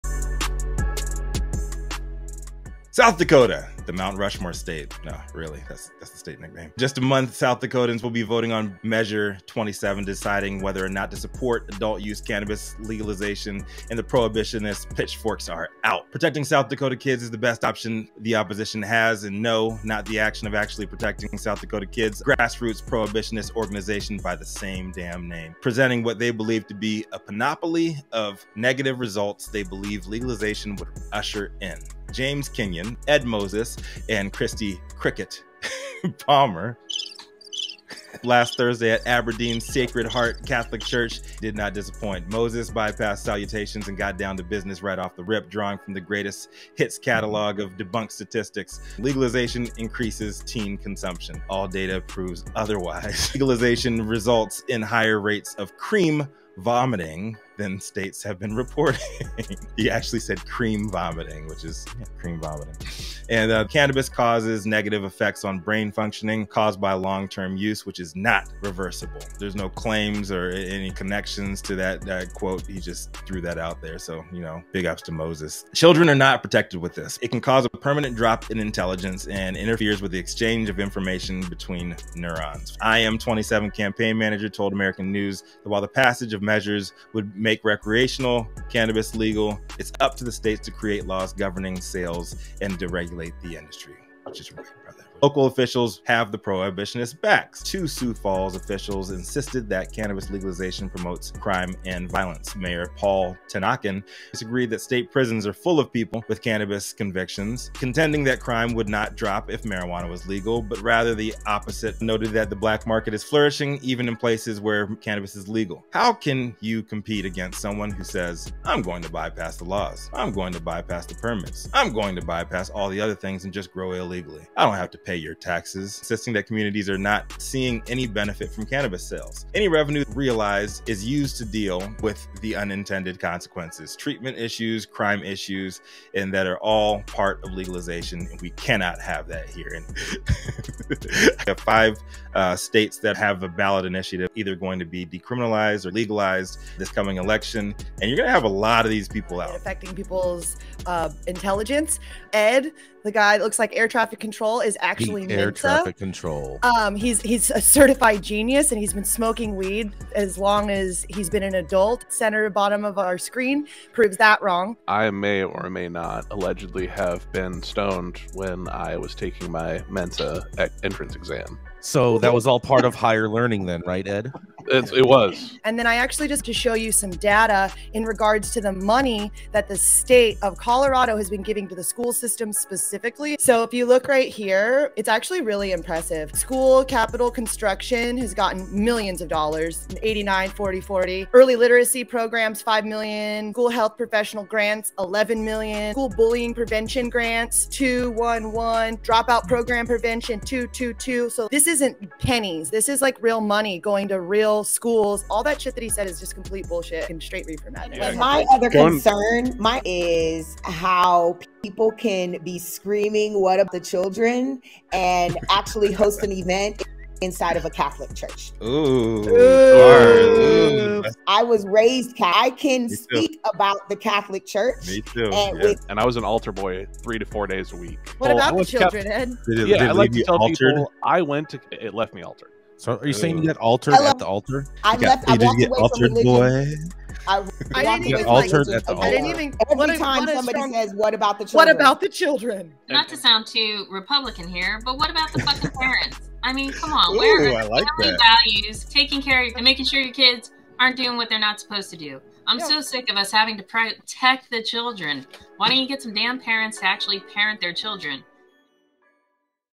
10/11/22 @highat9news Nugget: Can Prohibitionists Protect South Dakota Kids from Reefer Madness??
10/11 @highat9news beta 🗞️📰🚧
📺📡🔊Tune in LIVE on weekday mornings 9a PST/12p EST & join the conversation on your fave streaming platform found

For today’s headline, I covered the @Benzinga article by Nina Zdinjak: “Cannabis 'Can Cause A Permanent Drop In Intelligence' Claims Protecting South Dakota Kids Railing Against Legalization”

Grassroots prohibitionist org, Protecting South Dakota Kids presented all the negative things they believe cannabis legalization could bring, reciting all the debunked myths we’ve heard before and adding a new one to the mix, as well…